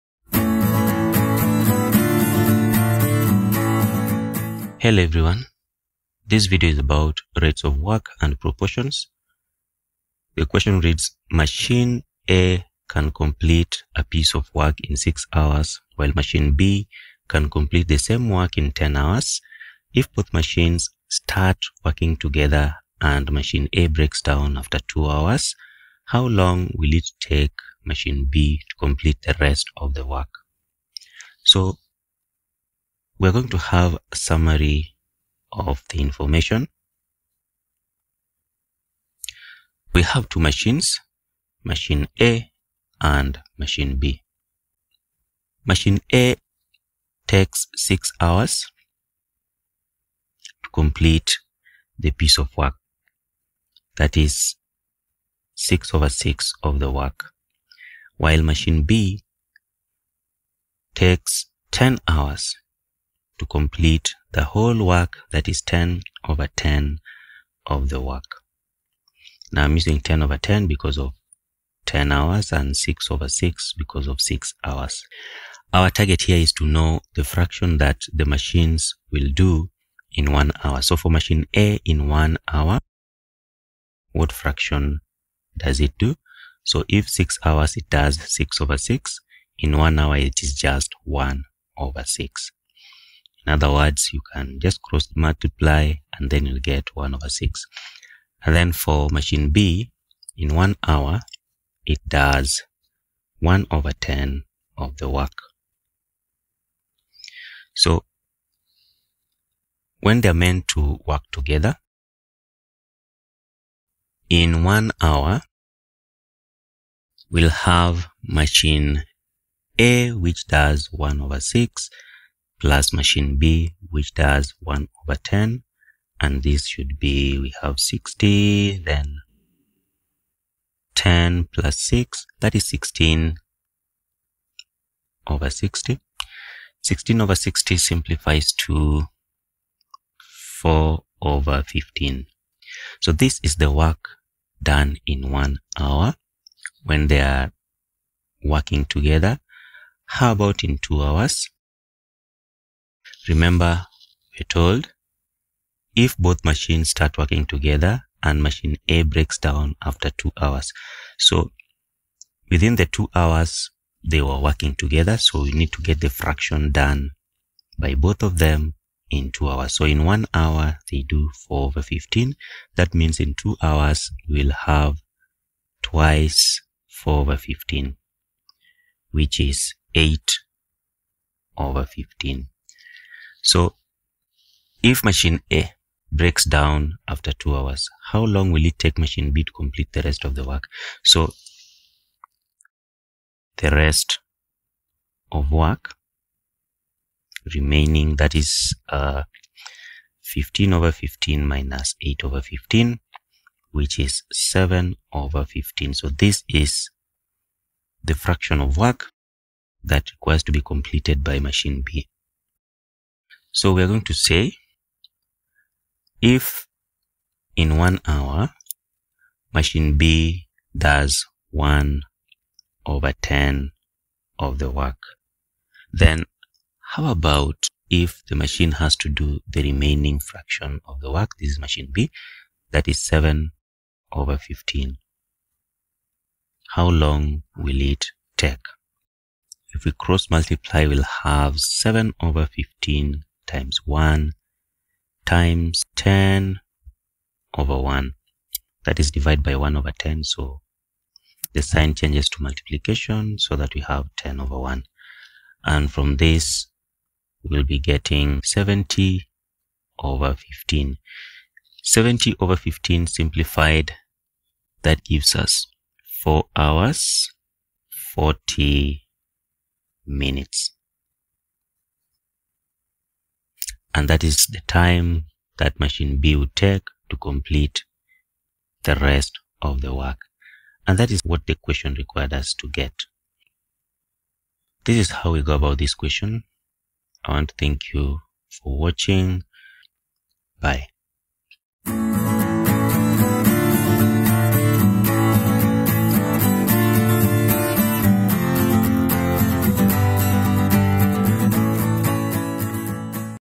FORM 3 MATHS: Rates of work and proportions - Problem Solved
In this video, we dive into Form 3 Maths and tackle rates of work and proportions with a detailed worked example. We solve a common problem where A can complete a piece of work in 6 hours while B can complete the same work in 10 hours. Join us as we break down the steps to solve this problem and gain a better understanding of rates of work and proportions. If you're looking to strengthen your math skills, this video is perfect for you!

QUESTION
Machine A can complete a piece of work in 6 hours while machine B can complete the same work in 10 hours. If both machines start working together and machine A breaks down after 2 hours, how long will it take machine B to compete the rest of the work?